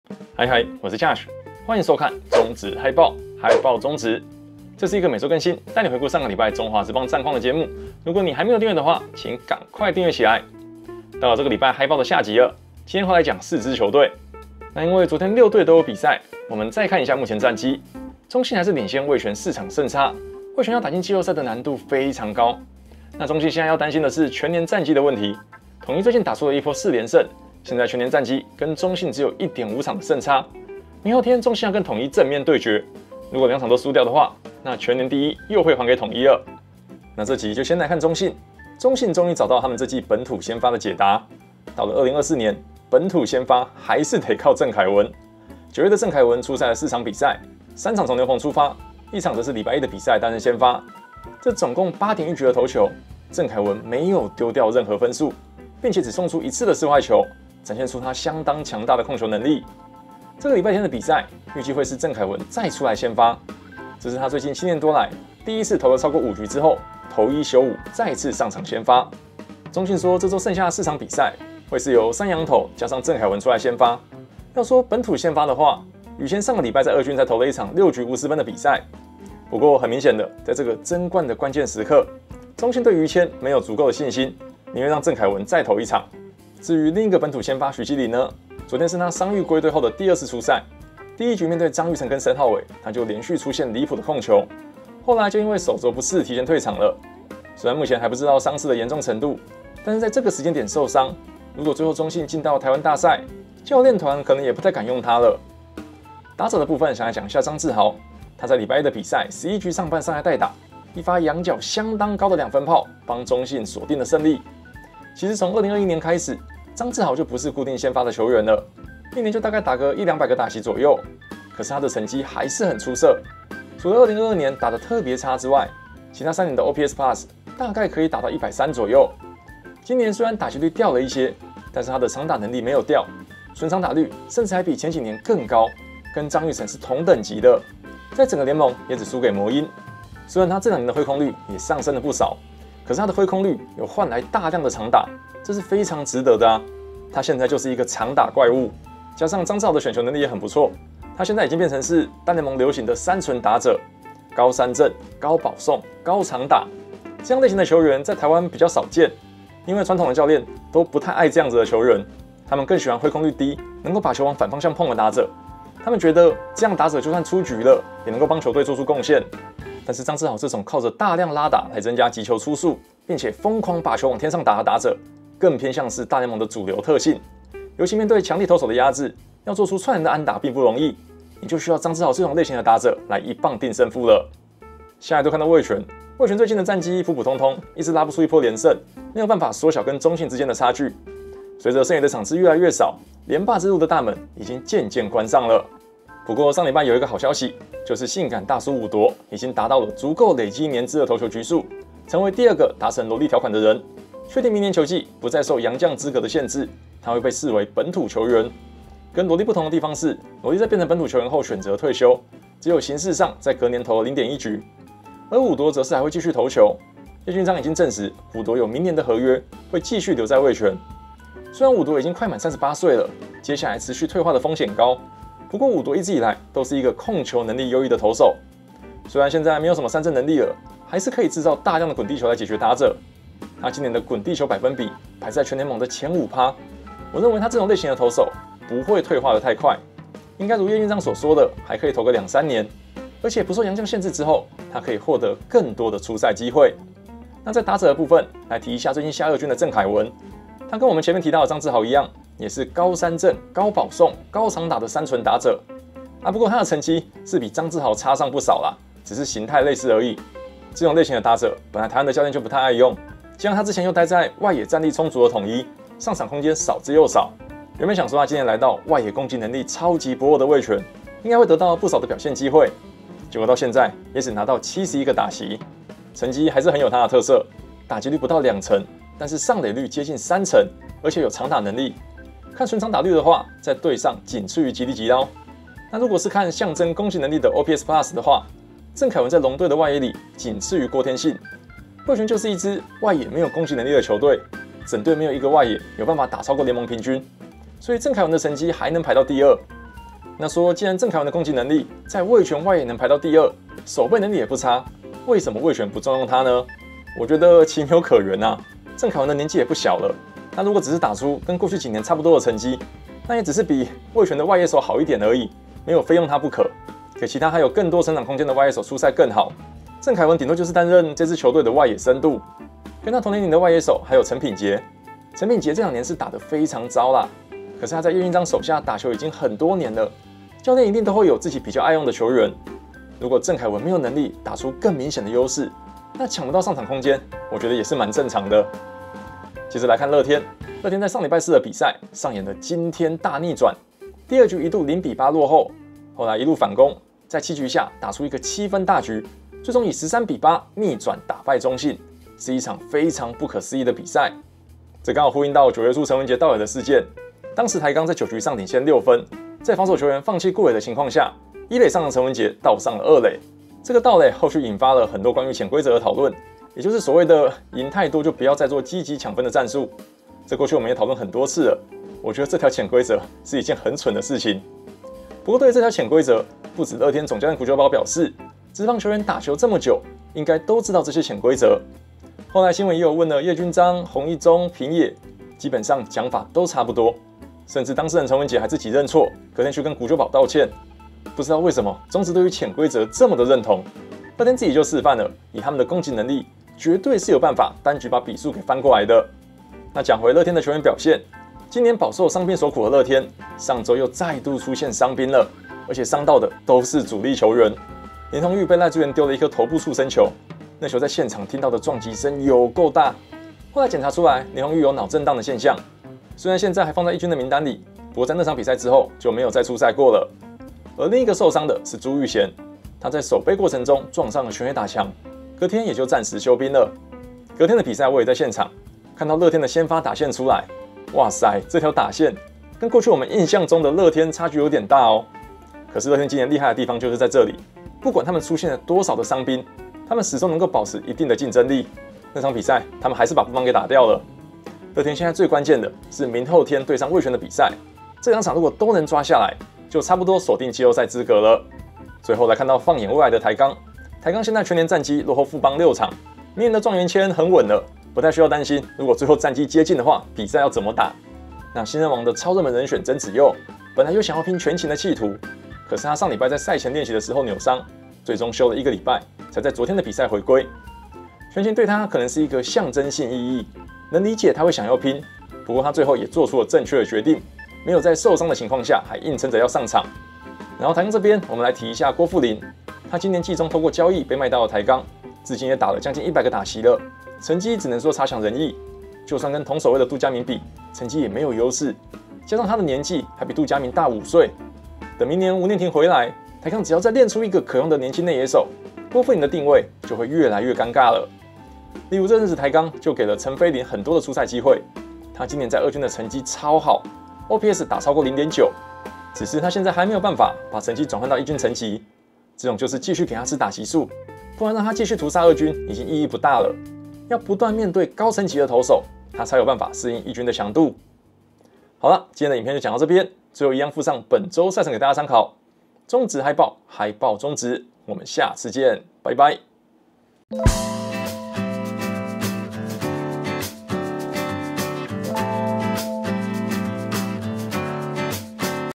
【中職嗨報2024 EP.23(下)】越老打越遠的長打怪物！跟張育成相提並論的長打能力！
我出的第一本書  嗨嗨，我是Josh！ 開賣啦

每個月75元小額贊助來幫助台南Josh頻道變得更好

工商邀約請寄到以下信箱